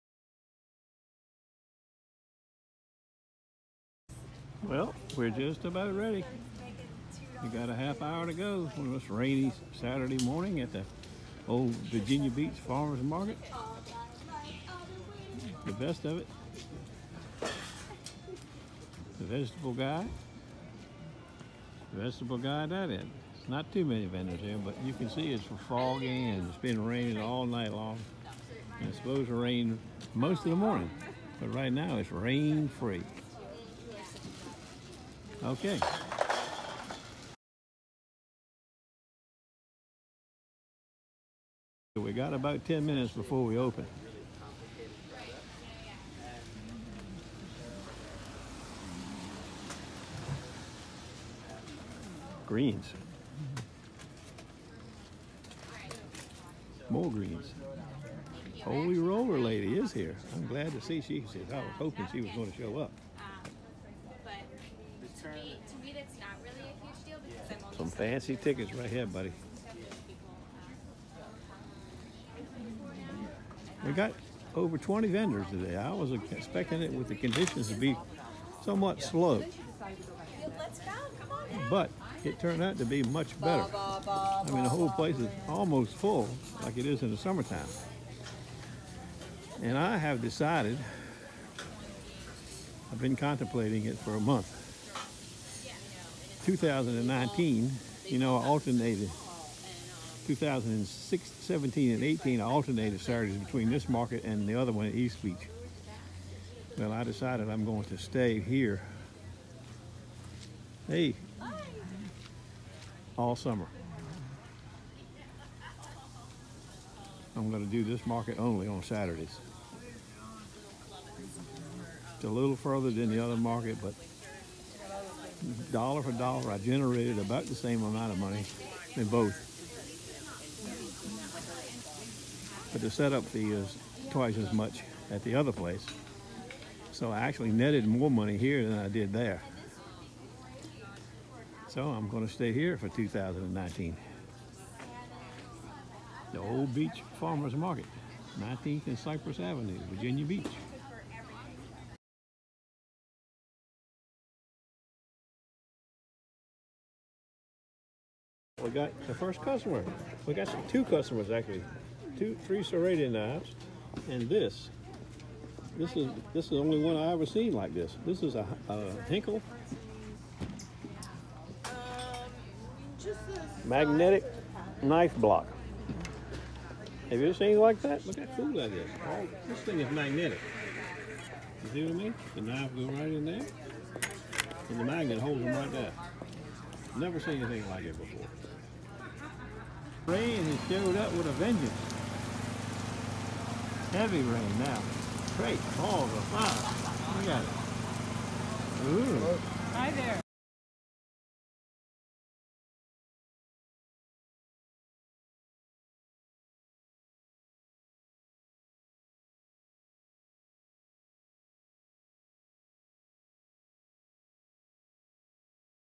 Old Beach Farmers Market - Sharpening event - saturday
weather was not the best - expected to be a bleak market day for knives, but it turned out pretty nice.
last market here until 2019 - Jan 19th.

good luck to everyone - hope to see you for surfchurch sunday

Stavrscoop
go pro hero session camera